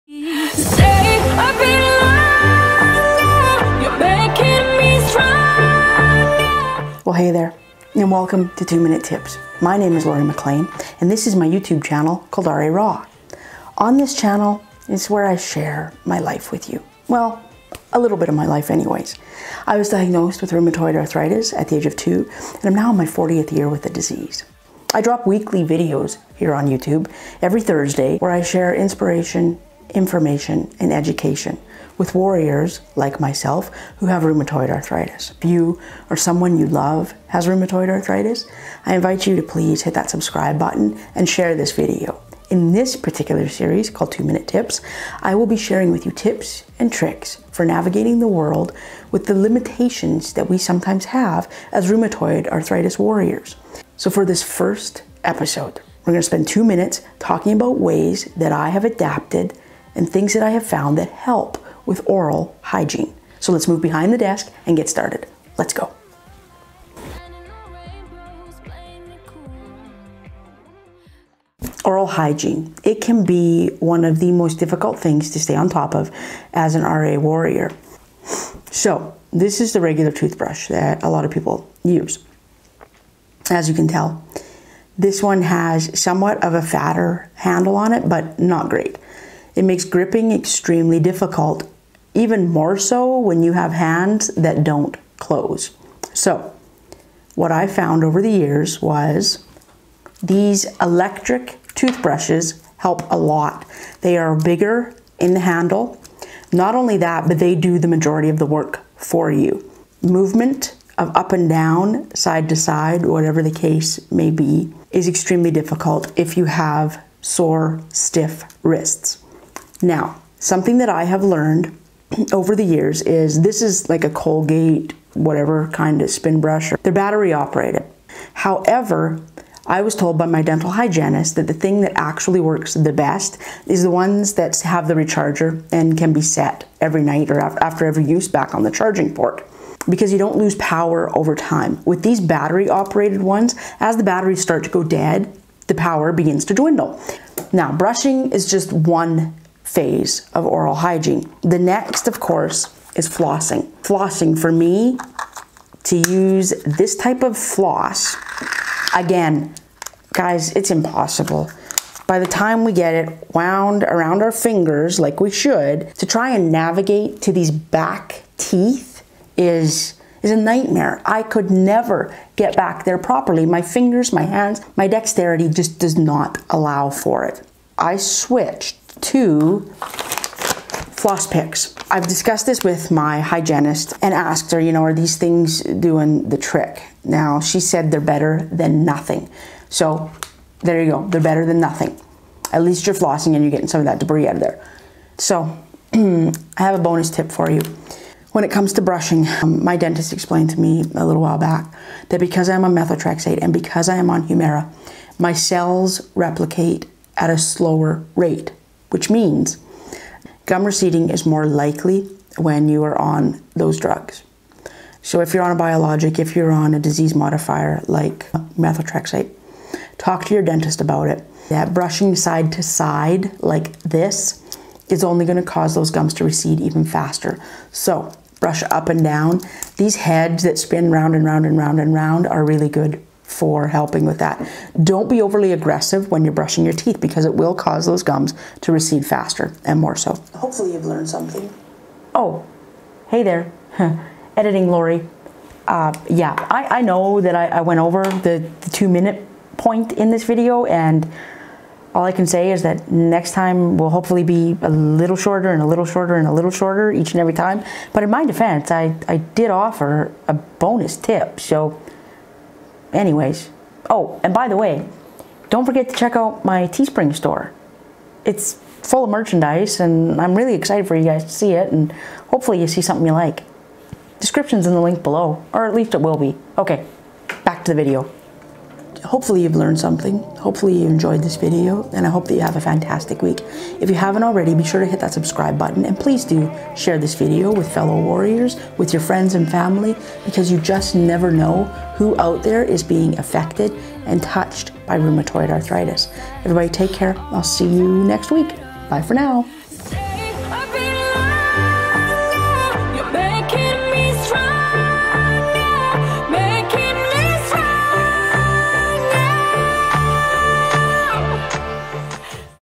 Two Minute Tip For Rheumatoid Arthritis Oral Hygiene
In this new weekly series, "Two Minute Tips" Lori MacLean shares her tips and tricks for navigating life with rheumatoid arthritis.

Lori MacLean was diagnosed at the age of 2 with juvenile rheumatoid arthritis and now in her 40th year with the disease she shares what she has learned from R.A.

MERCHANDISE